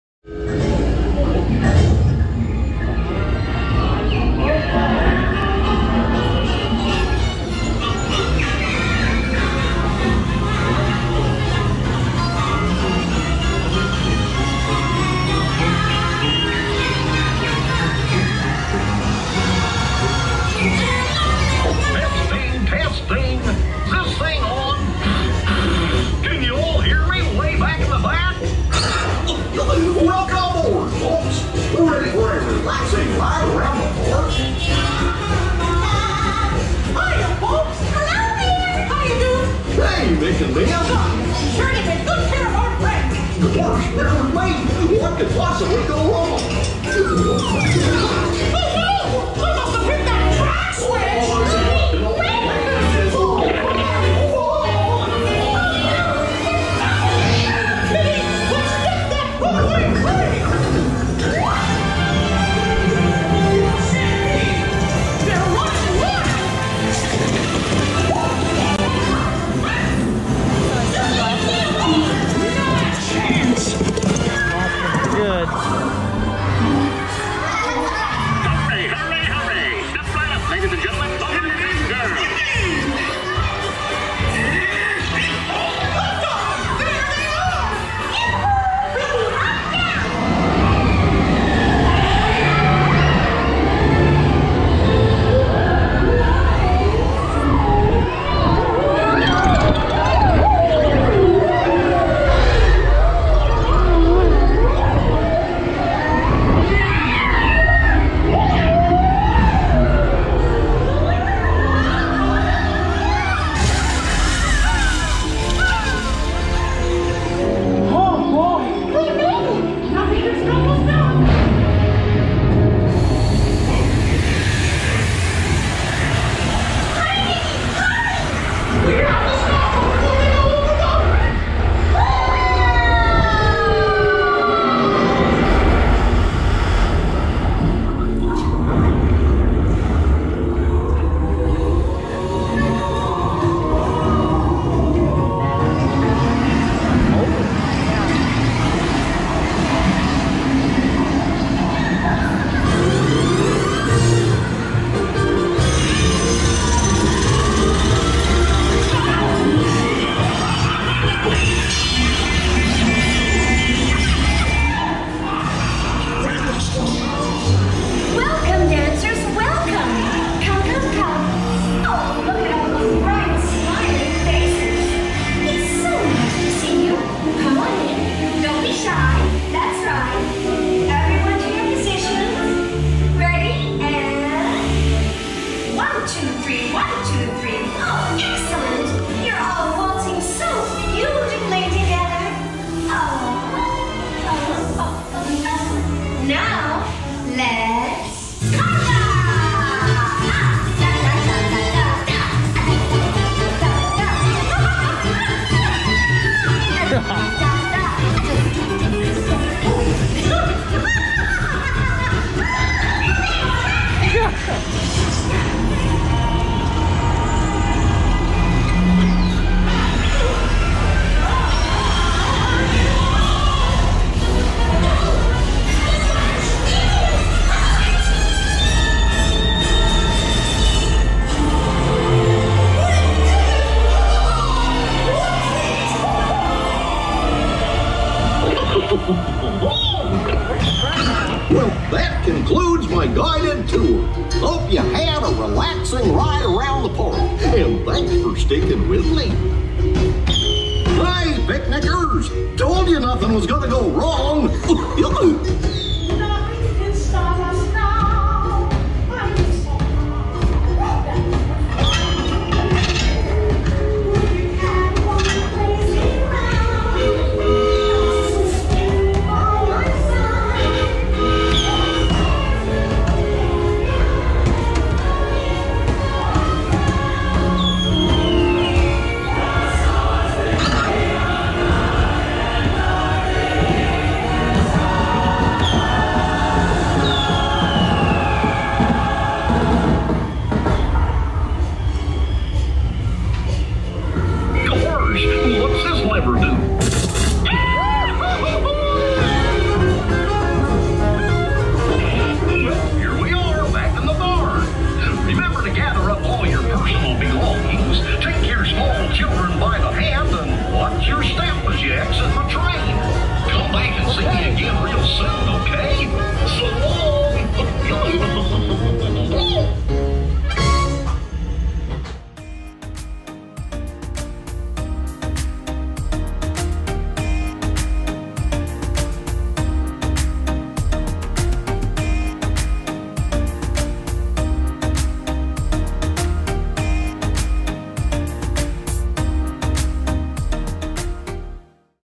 Mickey and Minnie's Runaway Railway | Hollywood Studios | i2i RIDE CAM
Ride along on Mickey and Minnie's Runaway Railway located in Hollywood Studios, part of Walt Disney World. Check out all sights and sounds of this Mickey and Minnie themed ride!